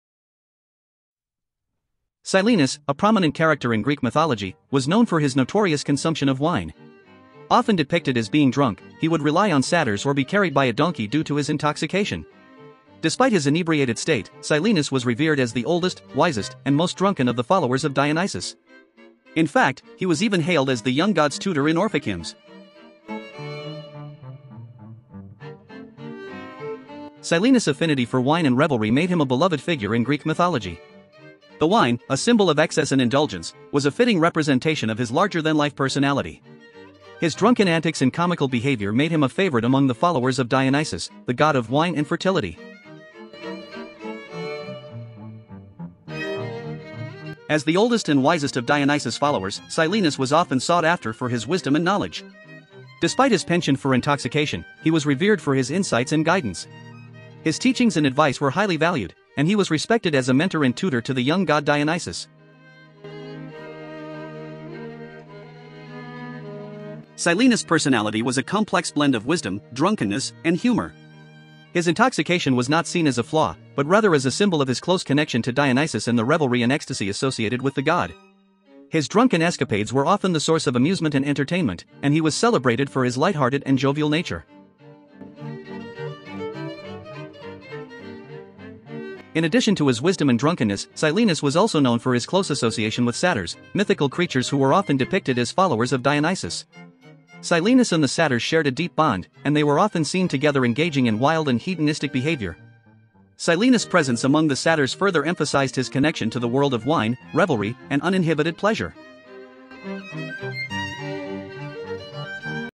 What is the personality of Silenus? Greek Mythology Story
Silenus, a prominent character in Greek mythology, was known for his notorious consumption of wine. Often depicted as being drunk, he would rely on satyrs or be carried by a donkey due to his intoxication. Despite his inebriated state, Silenus was revered as the oldest, wisest, and most drunken of the followers of Dionysus. In fact, he was even hailed as the young god's tutor in Orphic hymns.

Silenus' affinity for wine and revelry made him a beloved figure in Greek mythology. The wine, a symbol of excess and indulgence, was a fitting representation of his larger-than-life personality. His drunken antics and comical behavior made him a favorite among the followers of Dionysus, the god of wine and fertility.

As the oldest and wisest of Dionysus' followers, Silenus was often sought after for his wisdom and knowledge. Despite his penchant for intoxication, he was revered for his insights and guidance. His teachings and advice were highly valued, and he was respected as a mentor and tutor to the young god Dionysus.

Silenus' personality was a complex blend of wisdom, drunkenness, and humor. His intoxication was not seen as a flaw, but rather as a symbol of his close connection to Dionysus and the revelry and ecstasy associated with the god. His drunken escapades were often the source of amusement and entertainment, and he was celebrated for his lighthearted and jovial nature.

In addition to his wisdom and drunkenness, Silenus was also known for his close association with satyrs, mythical creatures who were often depicted as followers of Dionysus. Silenus and the satyrs shared a deep bond, and they were often seen together engaging in wild and hedonistic behavior. Silenus' presence among the satyrs further emphasized his connection to the world of wine, revelry, and uninhibited pleasure.

Despite his larger-than-life personality, Silenus was not without his flaws. His excessive drinking and revelry often led to chaotic and unpredictable behavior. He was known to be unpredictable and at times uncontrollable, adding an element of unpredictability to his already colorful persona.

In Greek mythology, Silenus' personality and behavior were symbolic of the duality of human nature. He embodied both wisdom and folly, sobriety and drunkenness, and seriousness and humor. His complex nature made him a multifaceted and intriguing character, revered for his wisdom and revered for his revelry.

Silenus' personality was a reflection of the larger themes and values of Greek mythology. His wisdom and drunkenness were symbolic of the dualities and contradictions that are inherent in human nature. As a mentor and tutor to Dionysus, he embodied the wisdom and guidance that were highly valued in Greek society.

In conclusion, Silenus' personality was a fascinating blend of wisdom, drunkenness, and humor. His larger-than-life persona made him a beloved and revered figure in Greek mythology, celebrated for his insights and revelry. He was a symbol of the duality of human nature, embodying both wisdom and folly, sobriety and drunkenness, and seriousness and humor. His complex personality and behavior made him a multifaceted and intriguing character, whose presence added depth and richness to the world of Greek mythology.